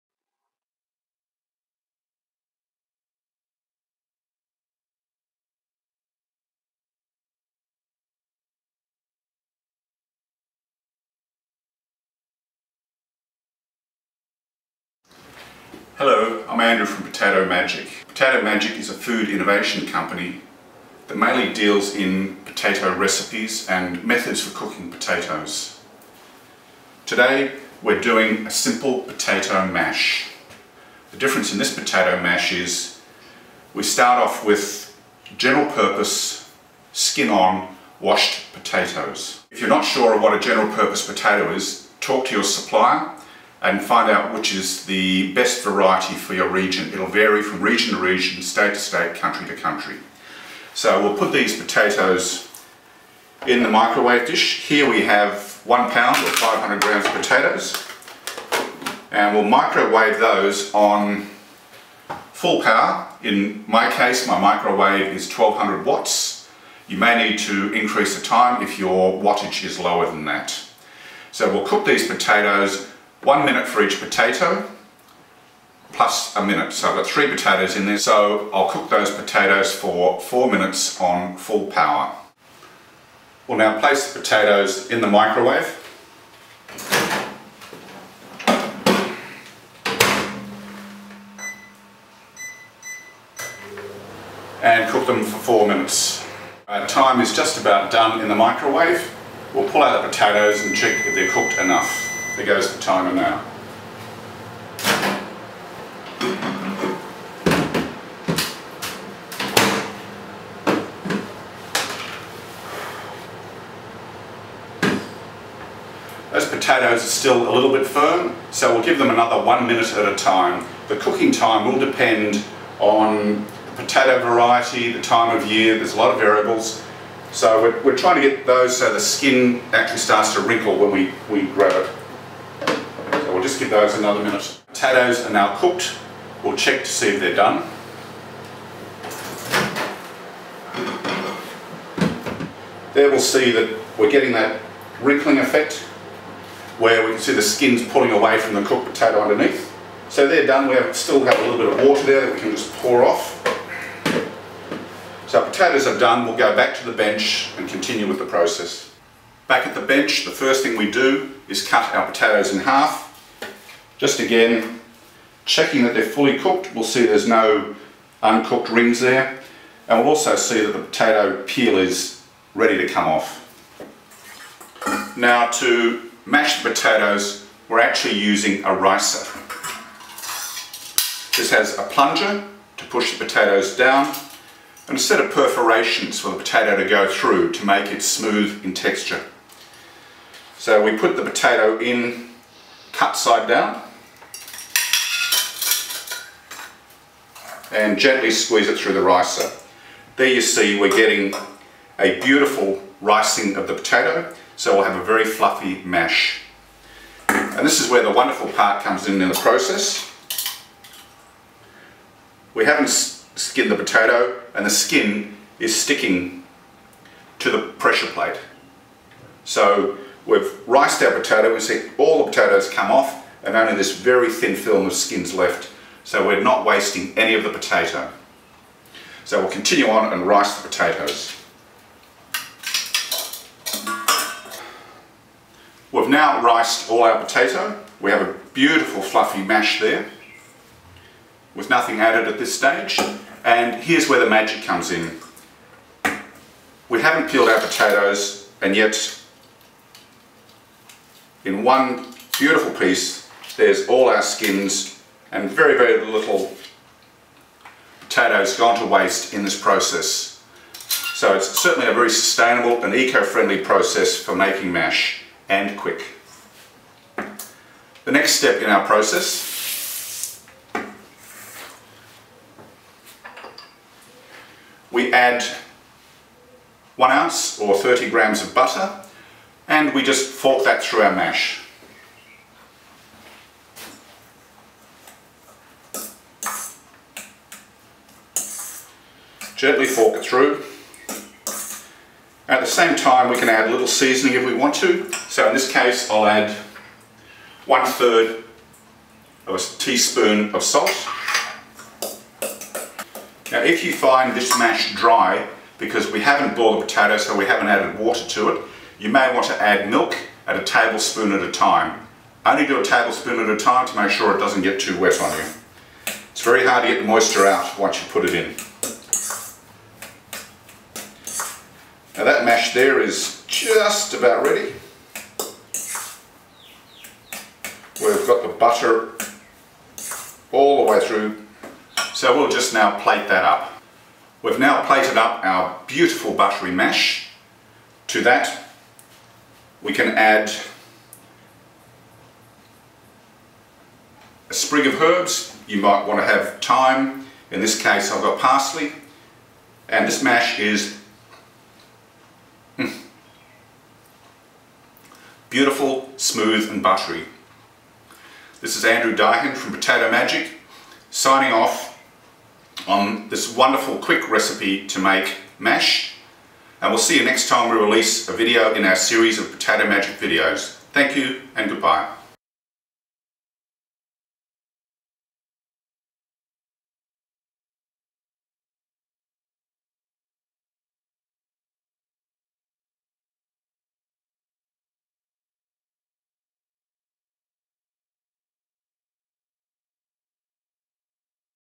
Delicious Mashed Potato in Under 8 Minutes
Check out this video to learn how to make delicious mashed potato in under 8 minutes without peeling or boiling the potatoes.